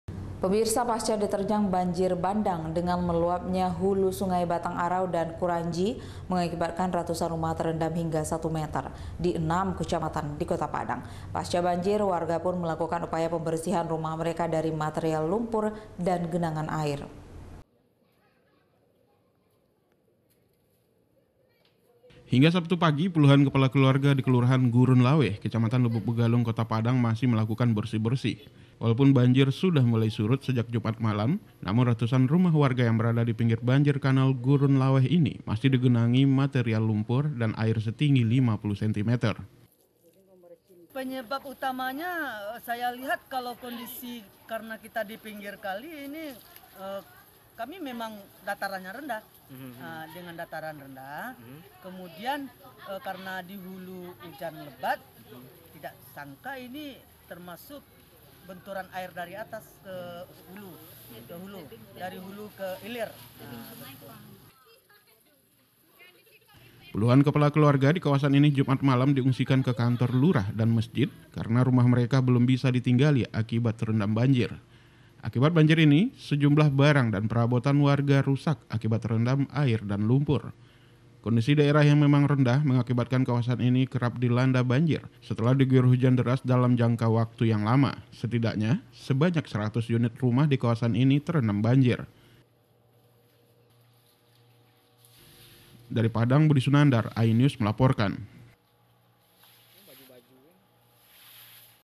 Pasca Banjir Warga Bersihkan Material Lumpur
Pasca diterjang banjir bandang, dengan meluapnya hulu sungai Batang Harau dan Kuranji, mengakibatkan ratusan rumah terendam hingga 1 meter di 6 Kecamatan. Wargapun masih melakukan upaya pembersihan rumah mereka, dari material lumpur dan genangan air.